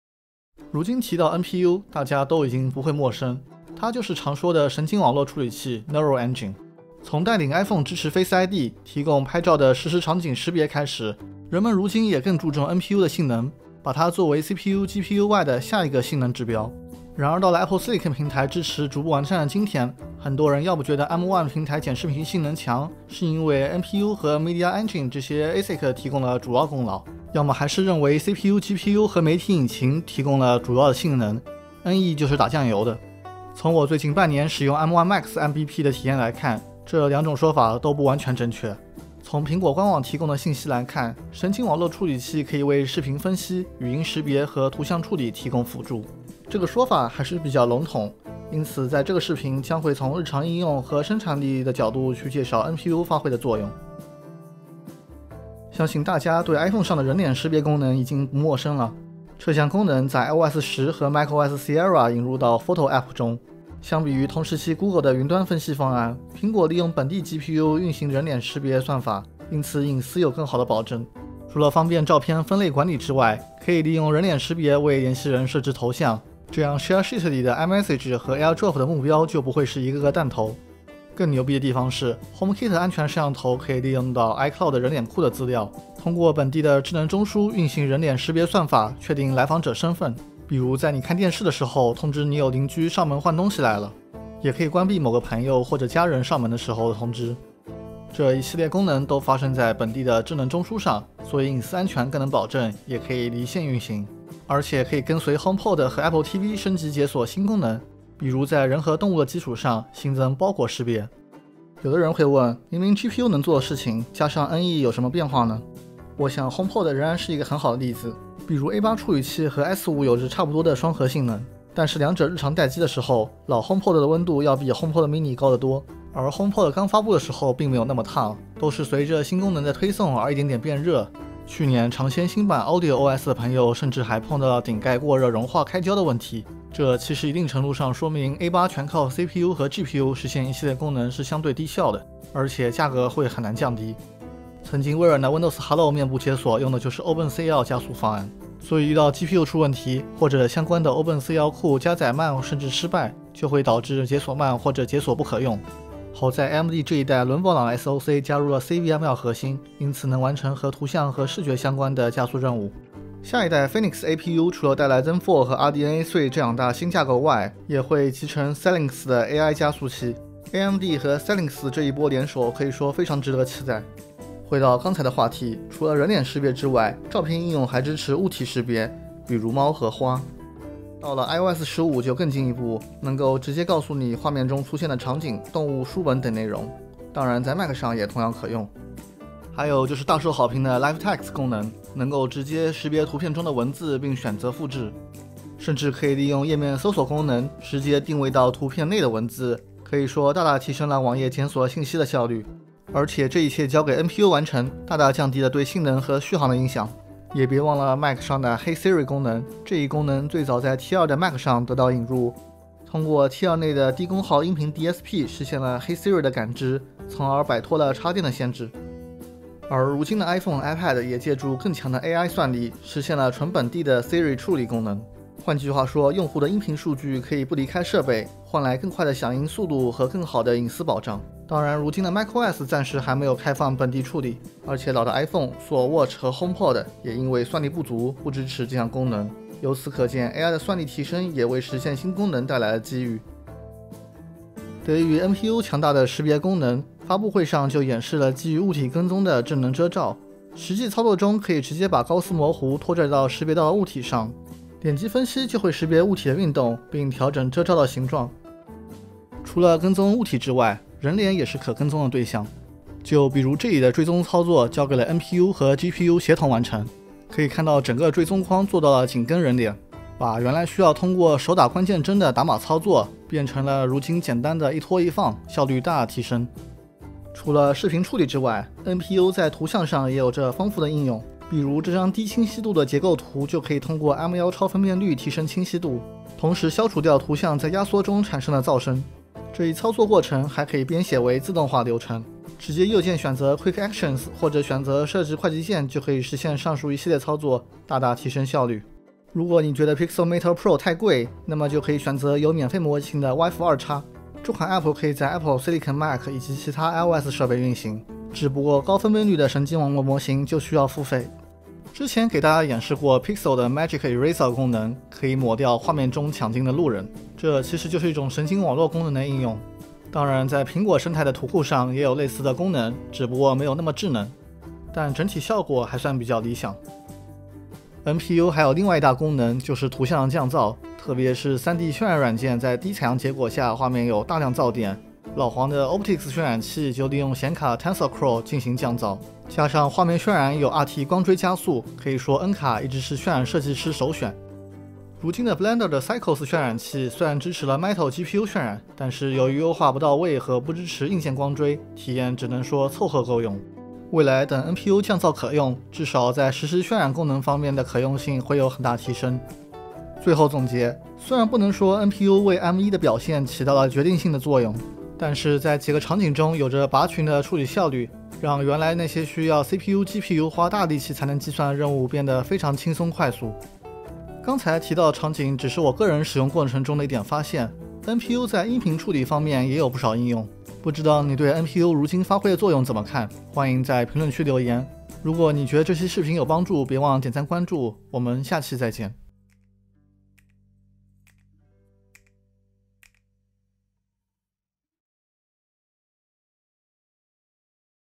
M1的NPU用不到? 教你利用NPU助推生产力!
Apple M1 系列强大而高效的 CPU GPU 一直是大家关注的焦点, 而 NPU 在一旁则显得有些尴尬. 然而有的人觉得 NPU 就基本打酱油的, 也有人觉得 NPU 是 M1 强大且与众不同的关键模块. 这两种说法都不全对, 今天就来解析下神经网络引擎在 Mac 和 iPhone 的使用场景中发挥了什么作用, 其中哪些可以对我们日常生活和工作带来显著的效率提升.

Apple Silicon Neural Engine

相关 App: Final Cut Pro X Photos Pixelmator Pro Safari Waifu2x Blender CoreML

0:00 - Intro
0:54 - 日常应用
4:03 - 生产力场景